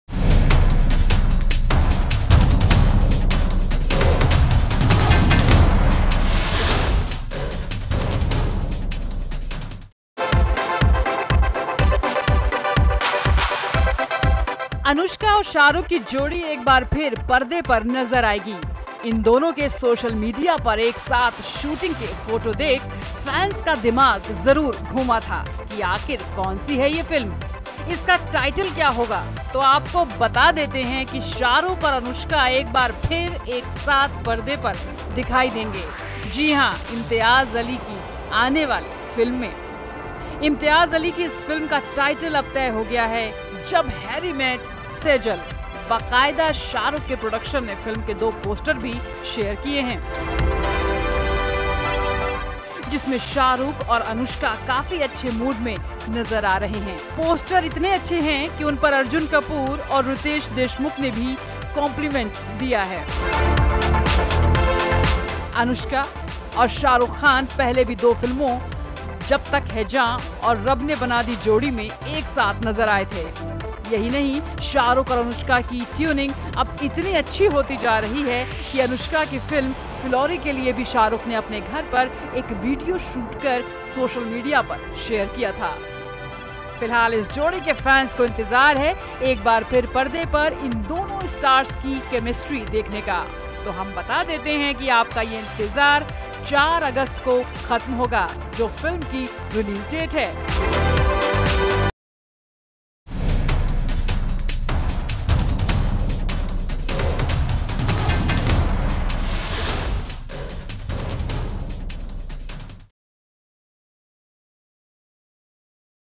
Jab Harry Met Sejal...Shahrukh Khan and Anushka upcomming movies...!!!
Finally, the title of Imtiaz Ali's film, starring Shah Rukh Khan and Anushka Sharma, has been revealed - it's Jab Harry Met Sejal. The title is quite similar to the director's 2007 film Jab We Met starring Shahid Kapoor and Kareena Kapoor Khan. Shah Rukh Khan's Red Chillies Entertainment, which has produced the film.